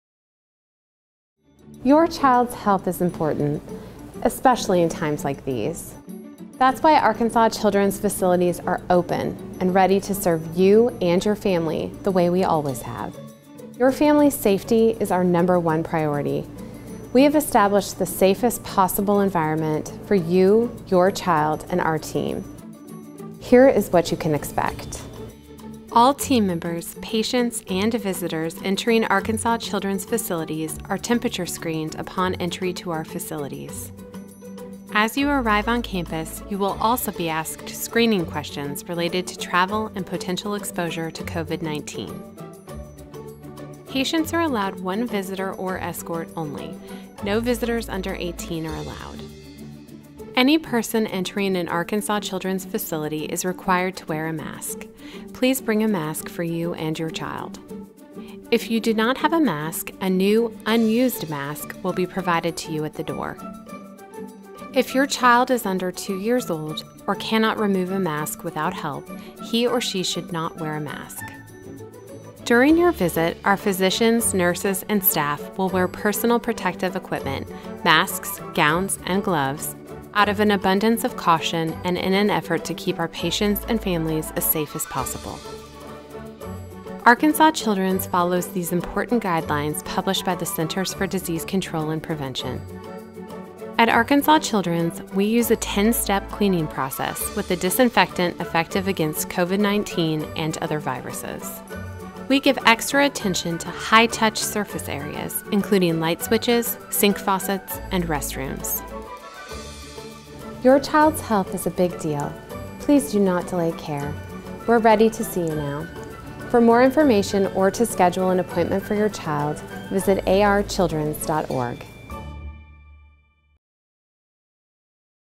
We're Ready to See You Now! What to Expect when Entering Arkansas Children's Facilities!
Your child's health is a big deal. Don't delay care! We're ready to see you now. We've made our locations the safest possible place for patients, families, and team members.